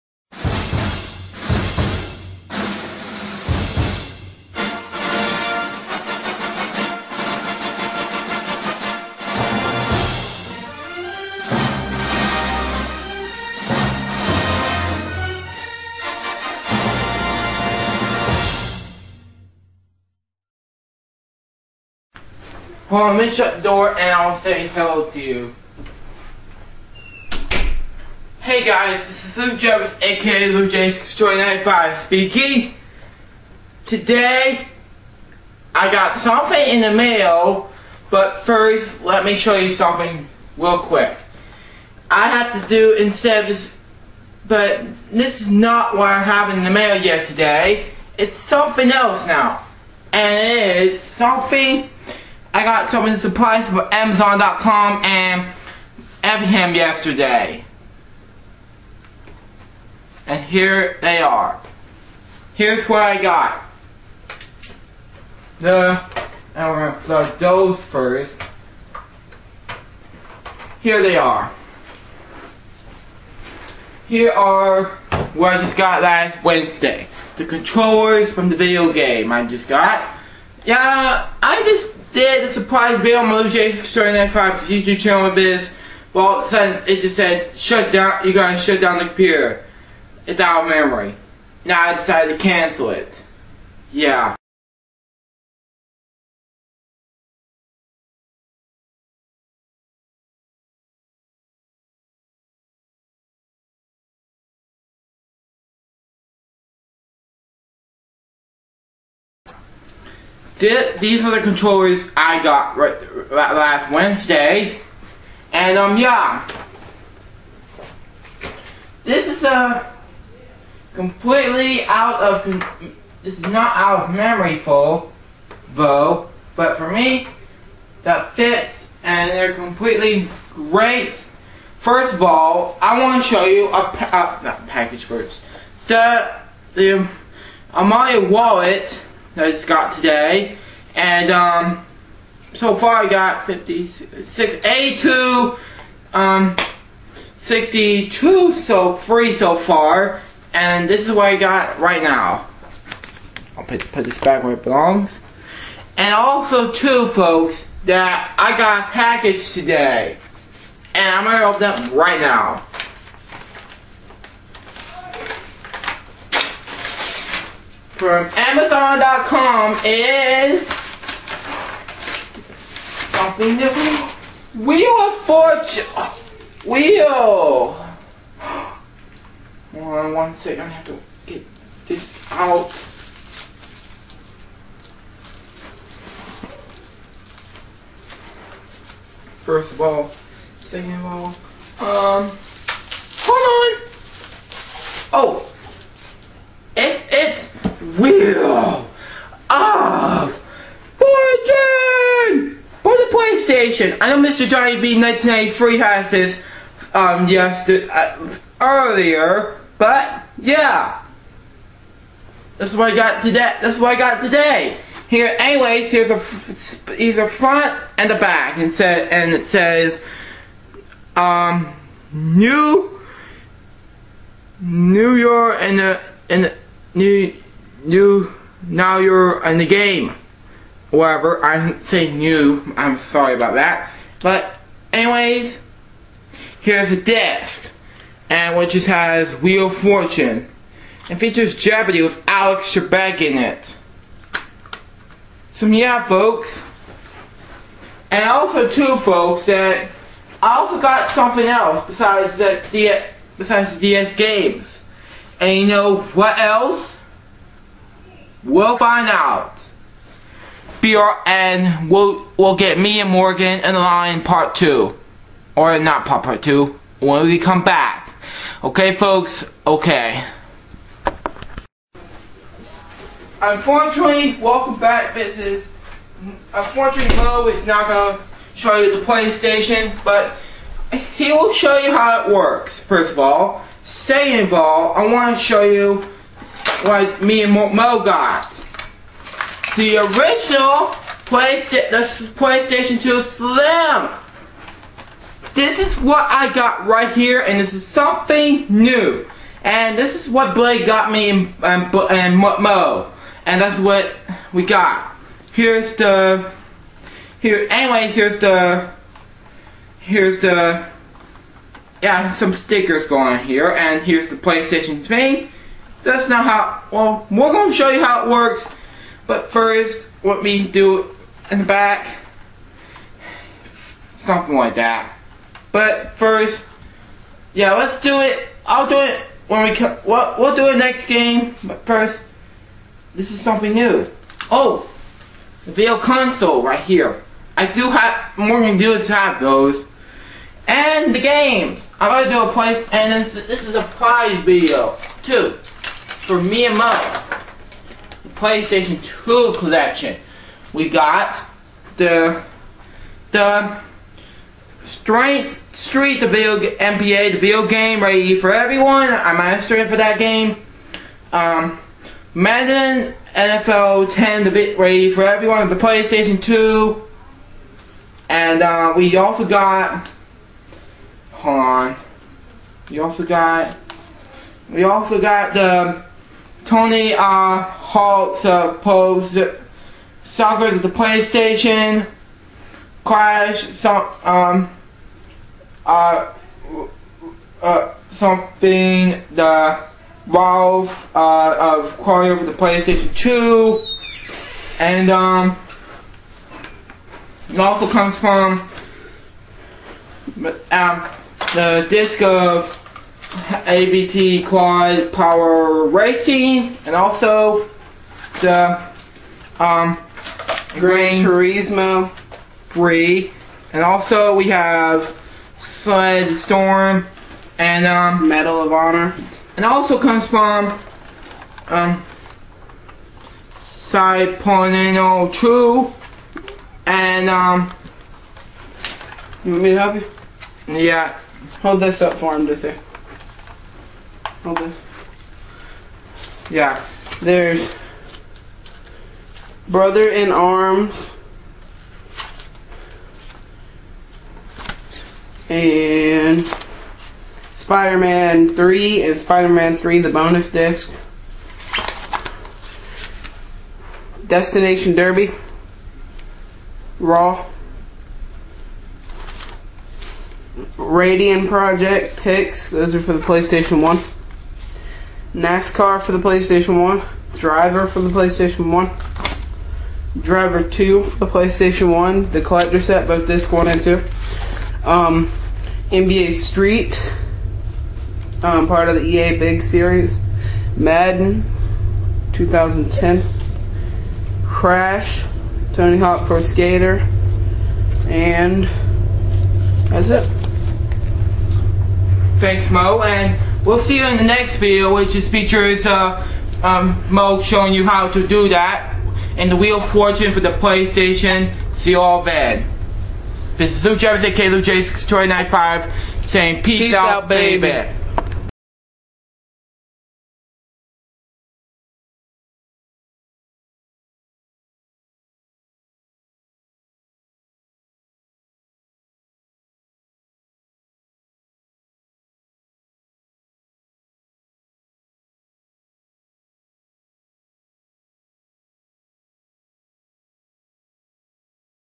LukeJ62895's Surprise Video #2 and My Surprises from My Early Birthday Party
I Got Wheel of Fortune from the PlayStation in the mail today which i know MrJohnnyV1993 just got that earlier and my AlumaWallet i just got from my early birthday party! Plus My Brother Morgan and I are gonna show you the PS2 Slim and the PS1 and PS2 games! Enjoy!